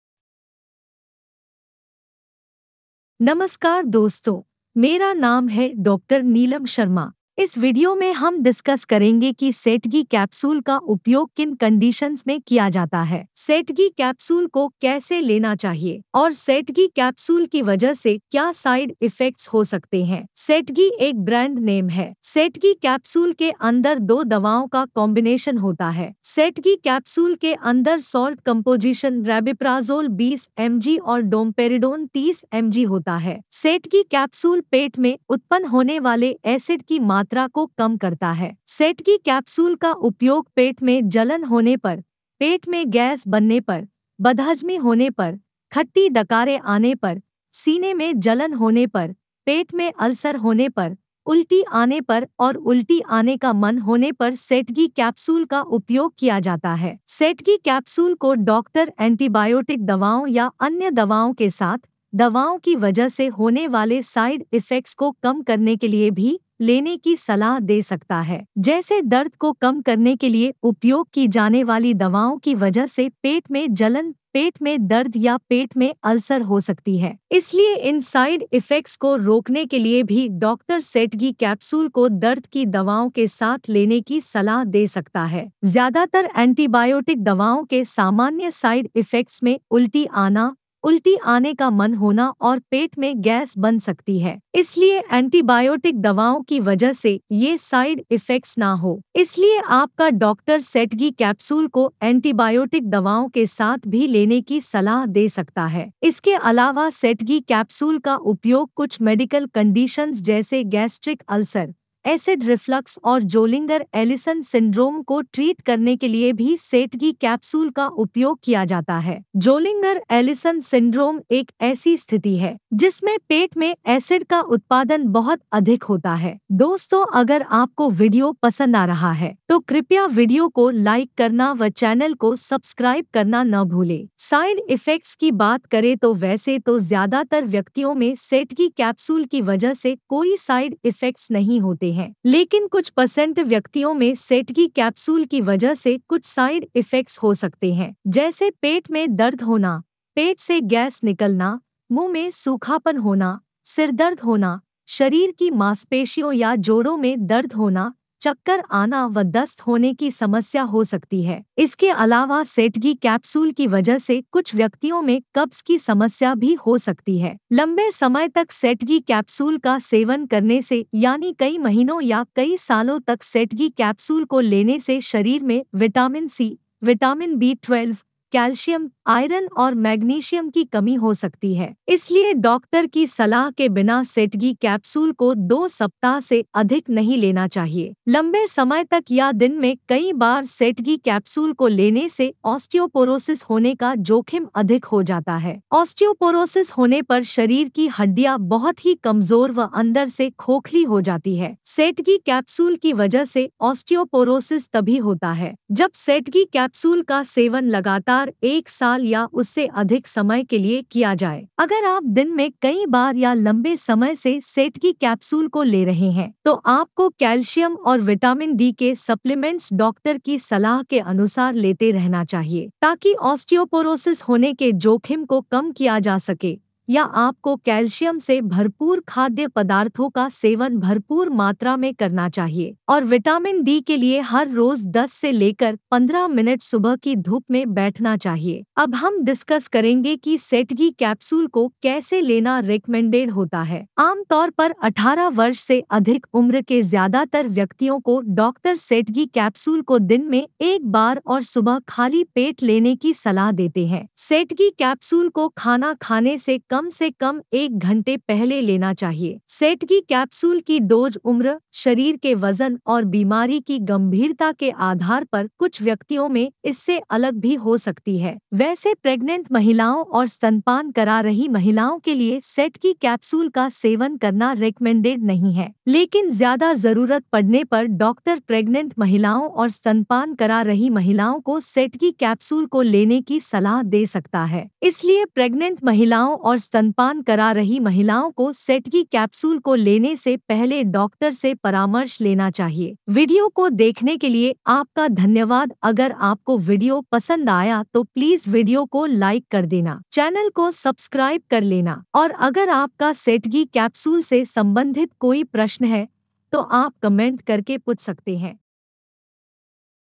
Setgi Capsule Uses & Side Effects | setgi capsule Ke Fayde Aur Nuksan
setgi capsule uses & side effects | setgi capsule ke fayde aur nuksan | how to use setgi capsule. in this video, we will discuss what are the uses and benefits of setgi capsule. How to use setgi capsule. We will also discuss what are the side effects of setgi capsule. Watch the complete video for detailed review of setgi capsule in hindi.